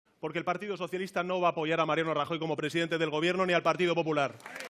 Pedro Sánchez: El PSOE no apoyará como presidente ni a Rajoy ni al PP
El PSOE no apoyará como presidente ni a Rajoy ni al PP, ni por activa ni por pasiva, el Secretario General del PSOE ha manifestado que descarta que gobierne el PP con la abstención del PSOE